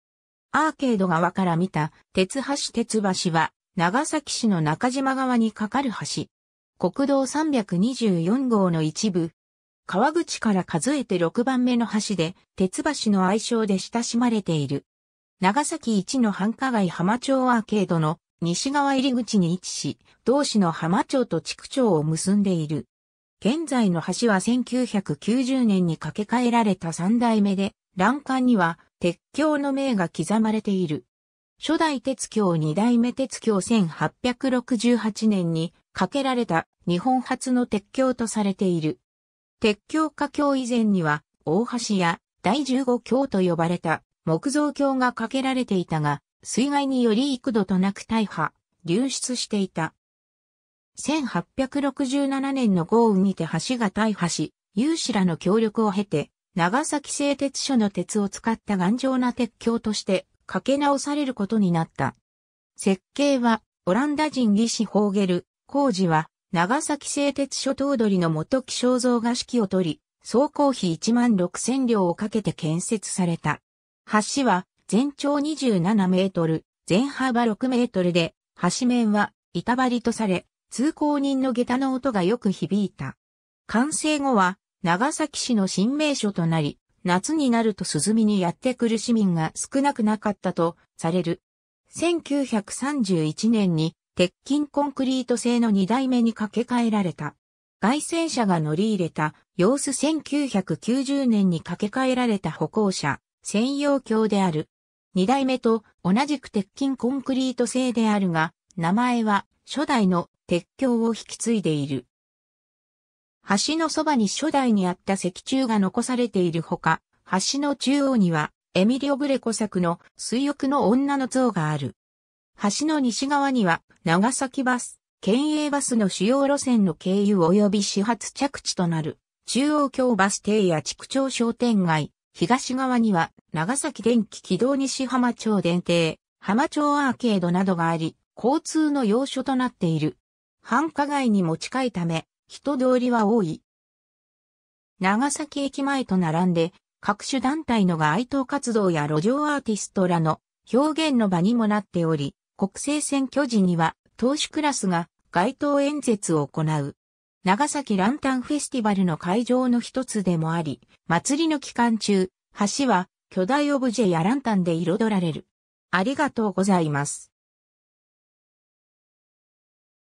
銕橋
アーケード側から見た銕橋 銕橋（くろがねばし）は、長崎市の中島川に架かる橋。
国道324号の一部。
 河口から数えて6番目の橋で、「てつばし」の愛称で親しまれている。
 長崎一の繁華街浜町アーケードの西側入り口に位置し、同市の浜町と築町を結んでいる。
現在の橋は1990年に架け替えられた3代目で、欄干には「鉄橋」の銘が刻まれている。
 初代銕橋（手彩色絵葉書） 2代目銕橋（彩色絵葉書） 1868年（明治元年）に架けられた、日本初の鉄橋とされている。
 鉄橋架橋以前には、「大橋」や「第十五橋」と呼ばれた木造橋が架けられていたが、水害により幾度となく大破・流失していた。
 1867年（慶応3年）の豪雨にて橋が大破し、有志らの協力を経て長崎製鉄所の鉄を使った頑丈な鉄橋として架け直されることになった。
設計はオランダ人技師ホーゲル、工事は長崎製鉄所頭取の本木昌造が指揮を採り、総工費1万6千両をかけて建設された。
 橋は全長27 m、全幅6 mで、橋面は板張りとされ、通行人の下駄の音がよく響いた。
完成後は長崎市の新名所となり、夏になると涼みにやって来る市民が少なくなかったとされる。
 1931年（昭和6年）に鉄筋コンクリート製の2代目に架け替えられた。
 街宣車が乗り入れた様子 1990年（平成2年）に架け替えられた歩行者専用橋である（構造的には車両乗り入れ可能だが、車両を乗り入れる場合は許可申請を必要とする）。
2代目と同じく鉄筋コンクリート製であるが、名前は初代の「鉄橋」を引き継いでいる。
橋のそばに初代にあった石柱が残されているほか、橋の中央にはエミリオ・グレコ作の『水浴の女』の像がある。
 橋の西側には長崎バス、県営バスの主要路線の経由および始発着地となる中央橋バス停や築町商店街、東側には長崎電気軌道西浜町（アーケード前）電停、浜町アーケードなどがあり、交通の要所となっている。
繁華街にも近いため、人通りは多い。
長崎駅前と並んで、各種団体の街頭活動や路上アーティストらの表現の場にもなっており、国政選挙時には党首クラスが街頭演説を行う。
 長崎ランタンフェスティバルの会場のひとつでもあり、祭りの期間中、橋は巨大オブジェやランタンで彩られる。